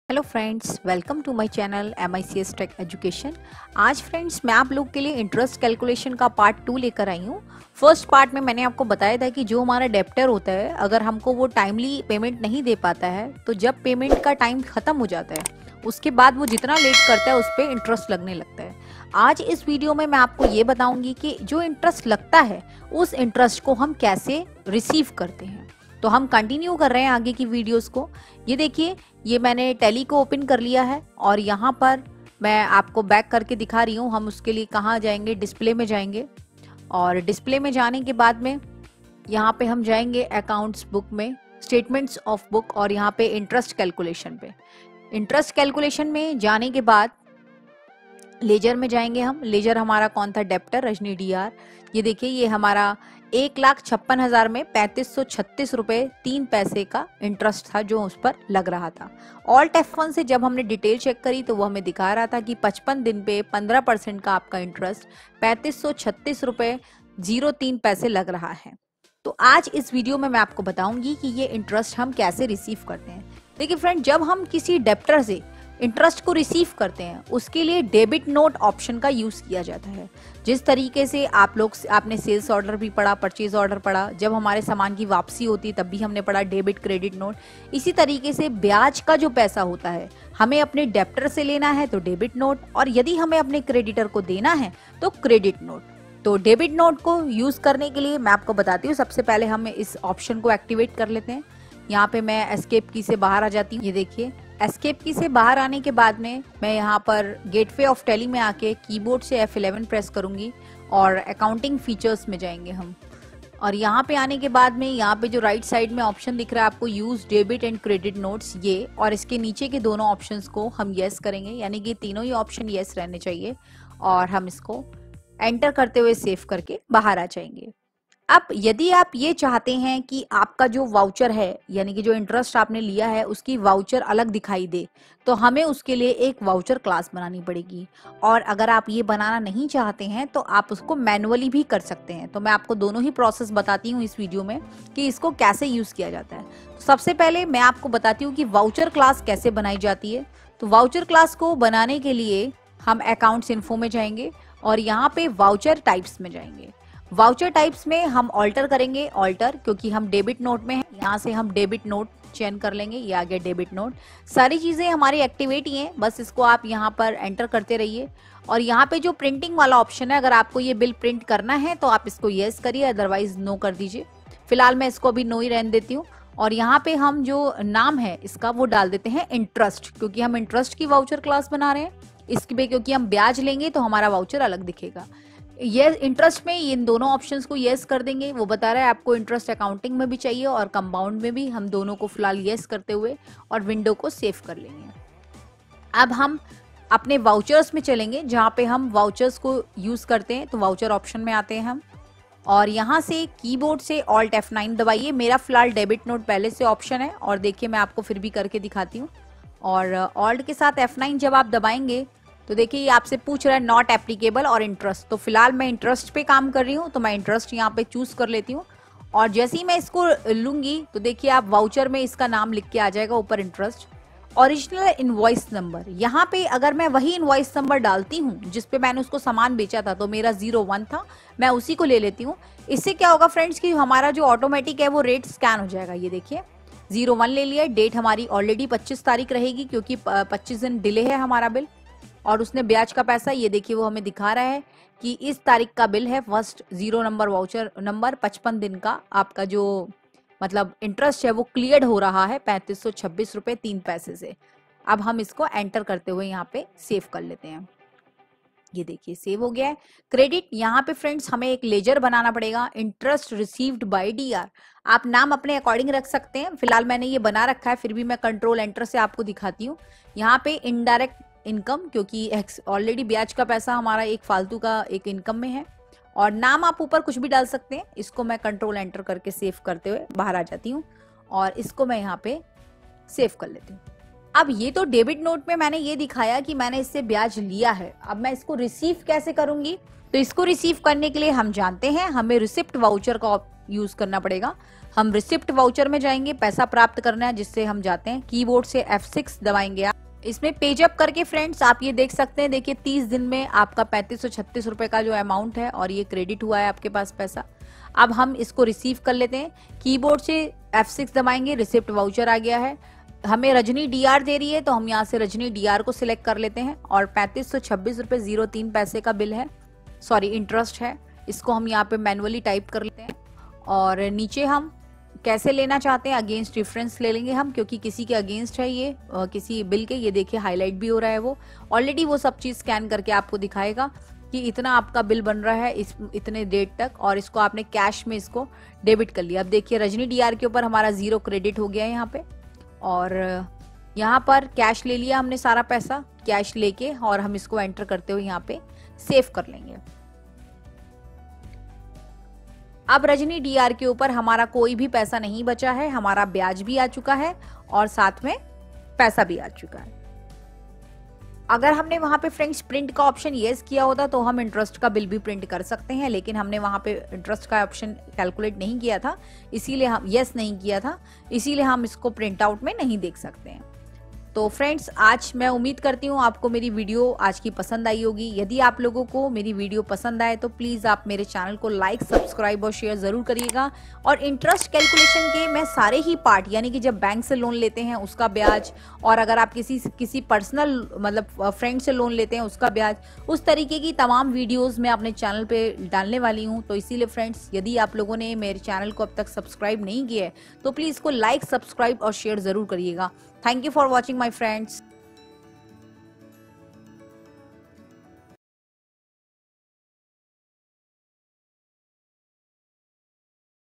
Intetest calculation in Tally ERP 9 | with full details and Entry| Tally ERP9 in hindi part-2
इस vedio में हमने interest कैलकुलेशन को कैसे रेचुरेसिएवे क्या जाता है बताया है
यदि हमारा debtor टाइम से पैसे न दे तो उसे ब्याज कैसे प्राप्त ककरे
ABOUT MY CHANNEL- MICS TECH EDUCATION IS A COMPUTER LEARNING YOUTUBE CHANNEL WHERE YOU WILL GET COMPUTER SOFTWARE VIDEOS IN HINDI